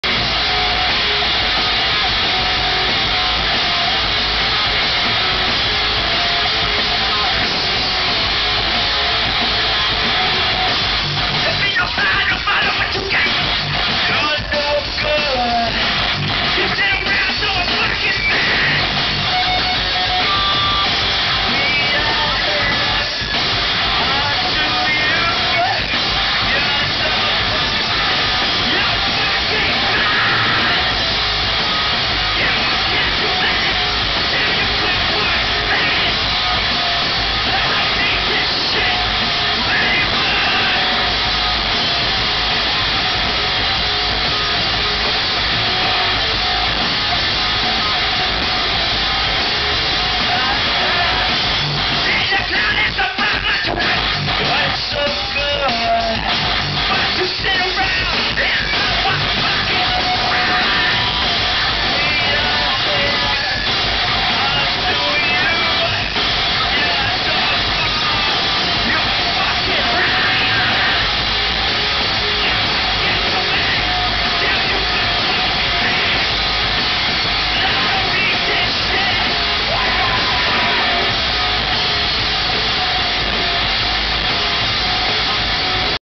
Deftones -- Live in Fresno
Deftones perform at Woodward Park Rotary Amphitheatre in Fresno, Calif. on 8/7/10.